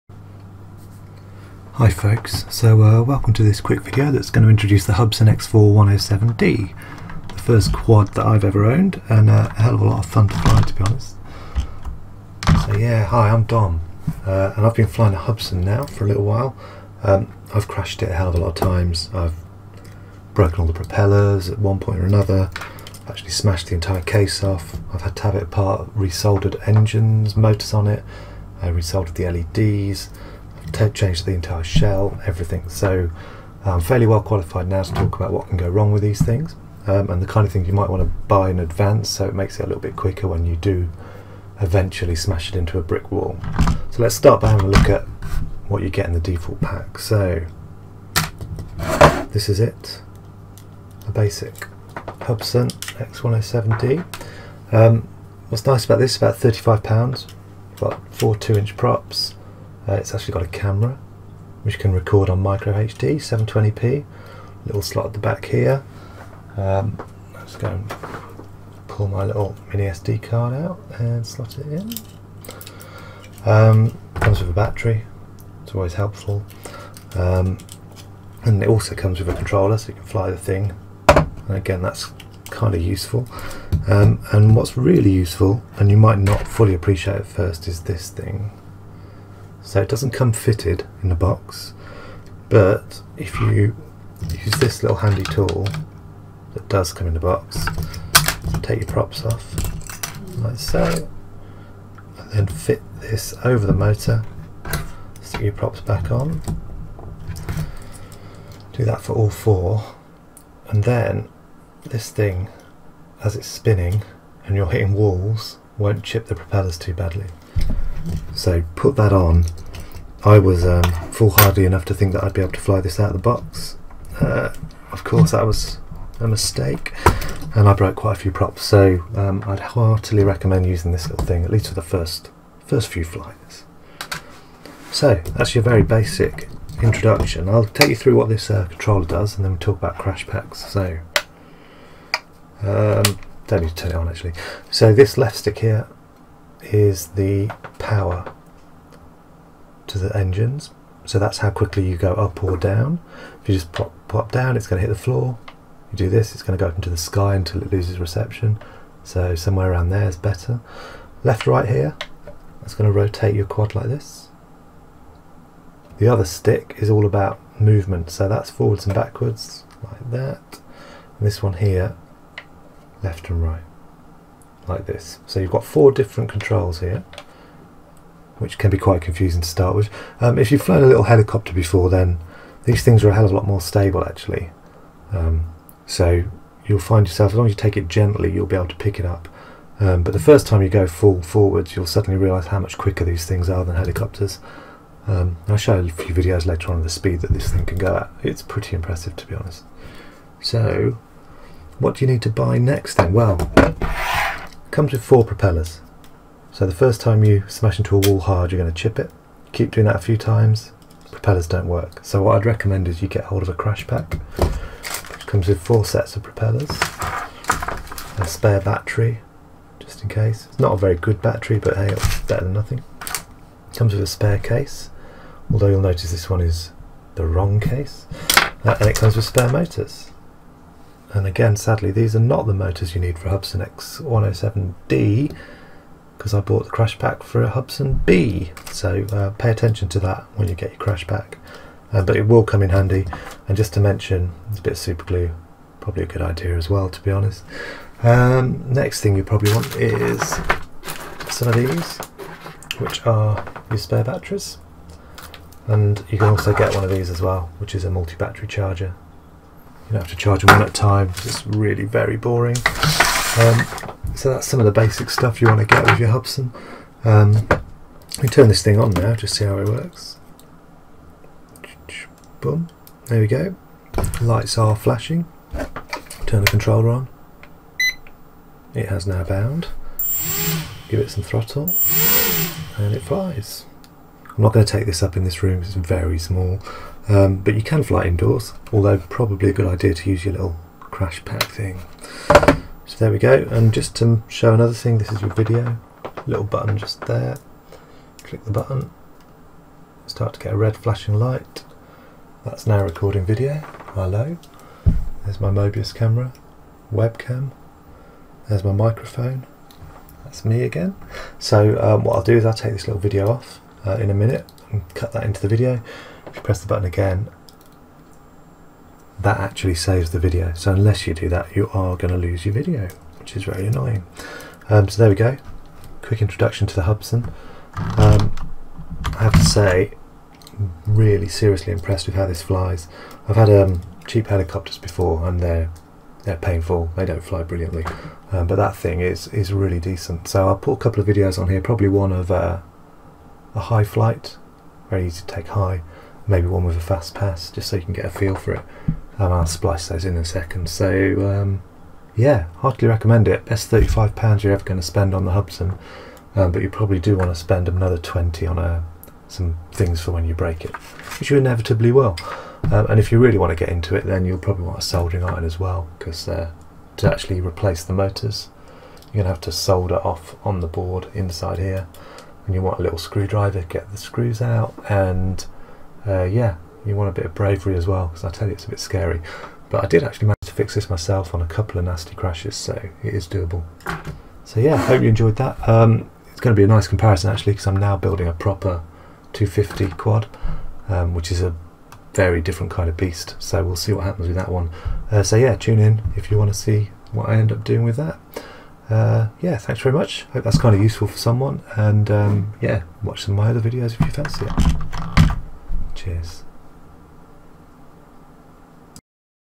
Review - Hubsan X4 107C Quadcopter
Introduction and review of the Hubsan X4 107C (note I call it the 107D incorrectly a few times...)

Pick these up from anywhere, other things you'll be needing are a 107C/D/L crash pack, and I'd recommend some more batteries too as flight time is fairly limited. The specific spares I've used since starting out...

4x Spare batteries + 4-way charger
4 sets of propellers
1 new motor
1 new shell (top and bottom)

Additionally I've soldered...
4x LEDs (because I didn't realise I didn't need to when changing the case...)
6x motors (4x when changing the case, 1x changing the motor to the wrong motor, 1x changing the motor to the right motor)